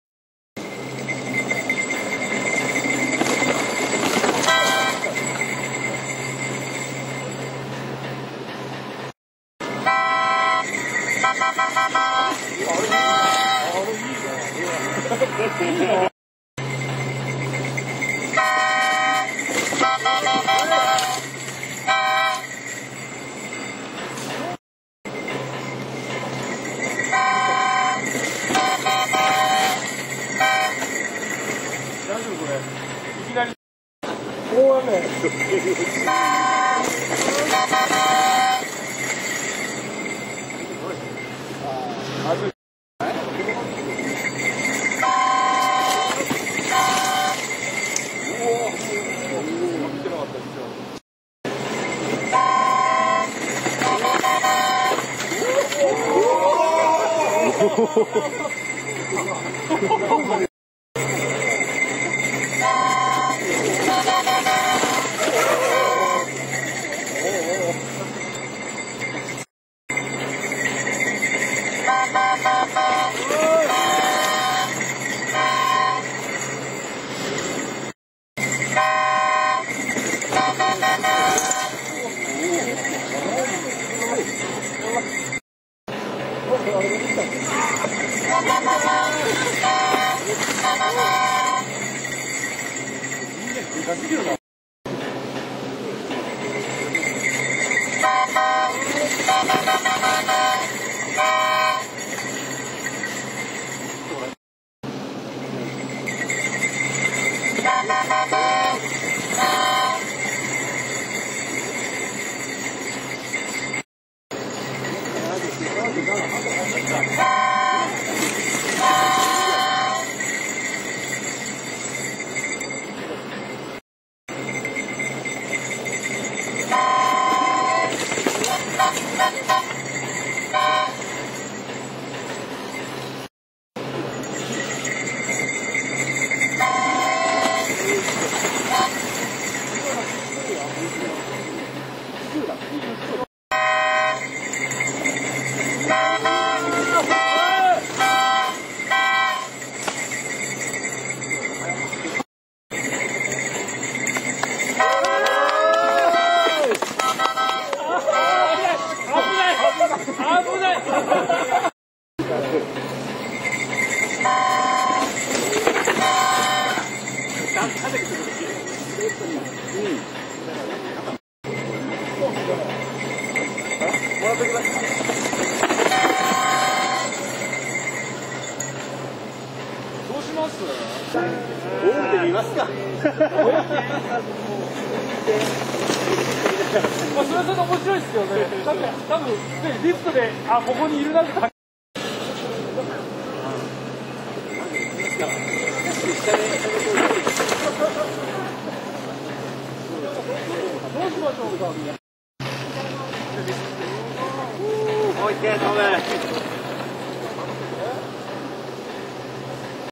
ze-f-2010-3.wmv
Japan National Mauntain Baike Championships 2010.July.17 Nagano/FUJIMI PANORAMA RESORT Senior/Elite Men Downhill クォリフィケイション-3